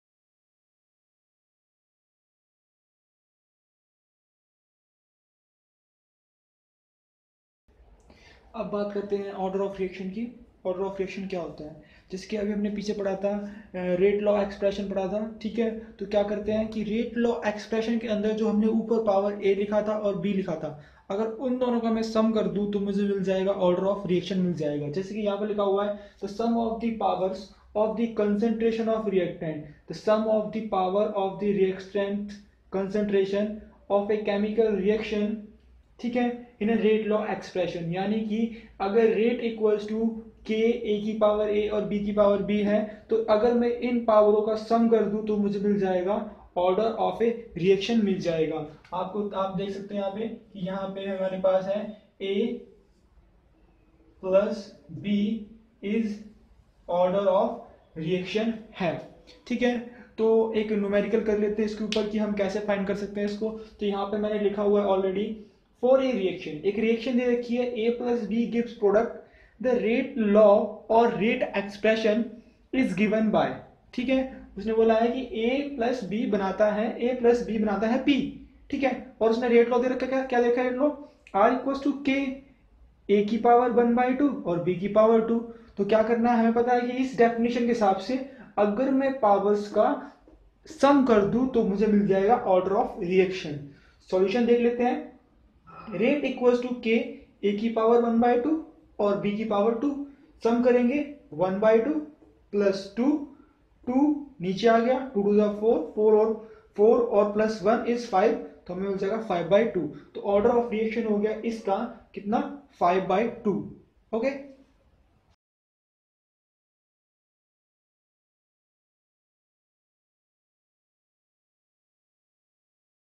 Chemical Kinetics Part 2 (Order of Reaction ) CBSE Class 12 NCERT
Chemistry Chemical Kinetics Part 2 CBSE Class 12 NCERT .

Chemical kinetics is the study and discussion of chemical reactions with respect to reaction rates, effect of various variables, re-arrangement of atoms, formation of intermediates etc. There are many topics to be discussed, and each of these topics is a tool for the study of chemical reactions.